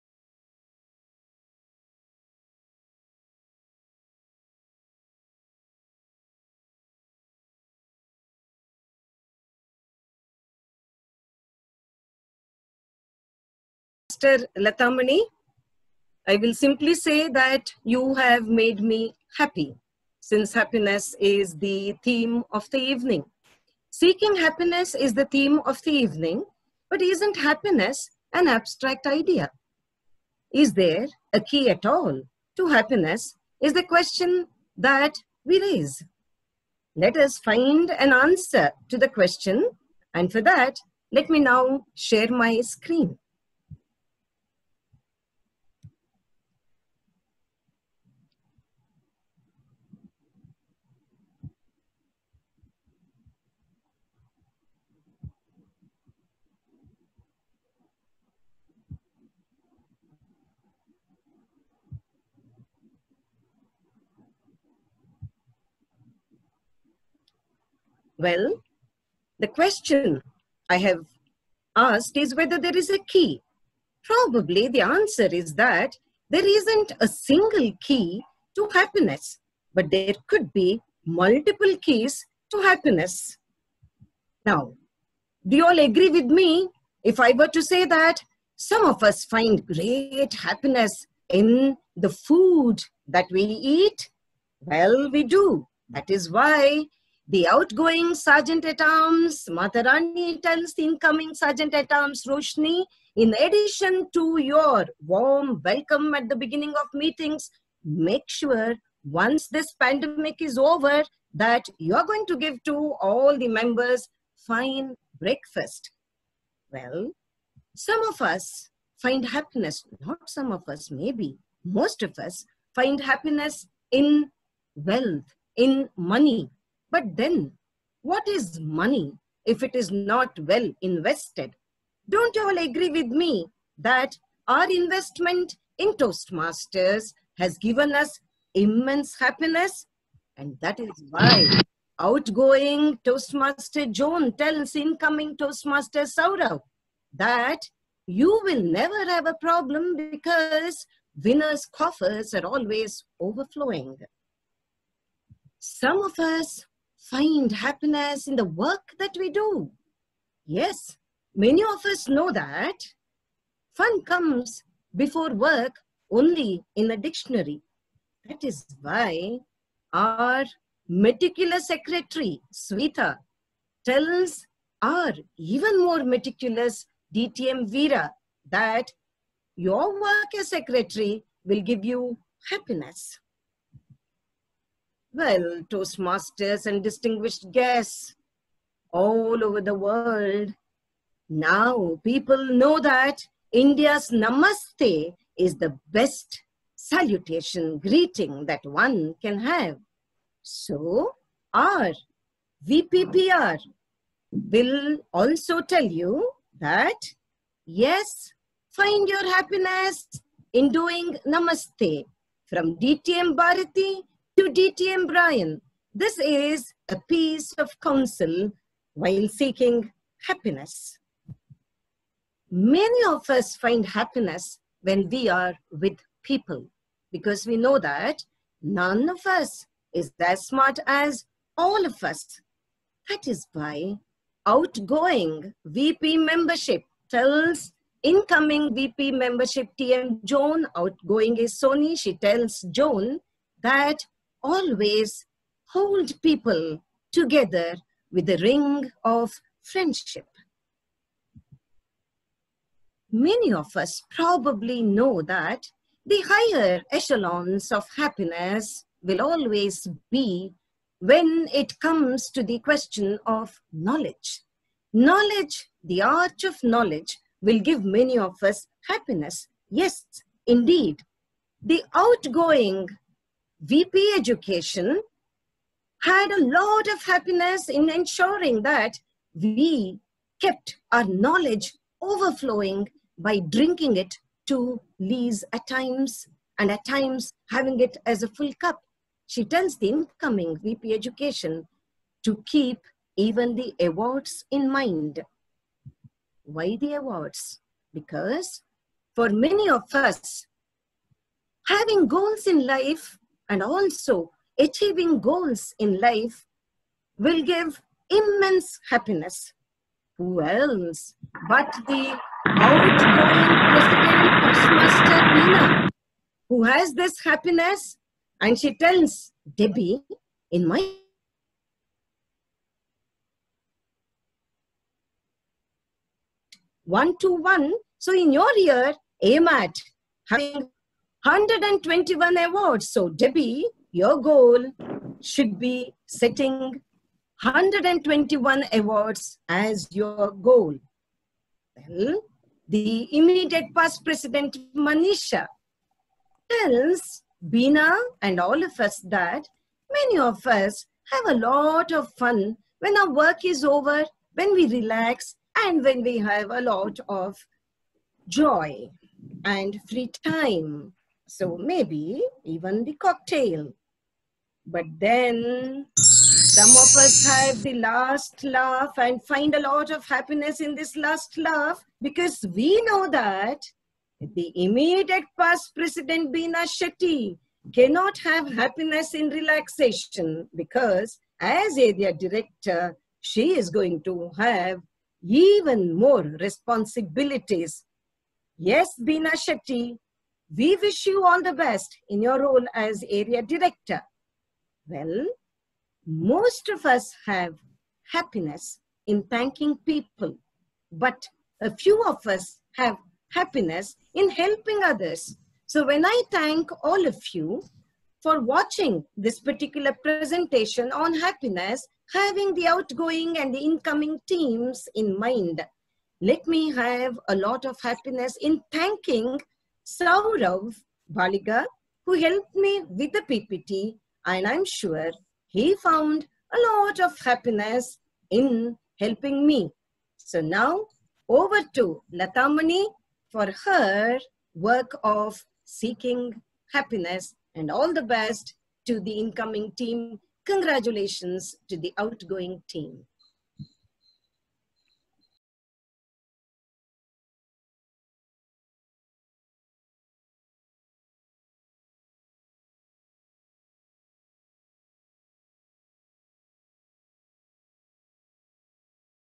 Installation Of Winners Toastmasters Club-2020THEMATIC HANDOVER BY DTM MALINI HEBBAR
Winners Toastmasters Club Mangalore Had Their Installation(Virtual) on July 5th 2020.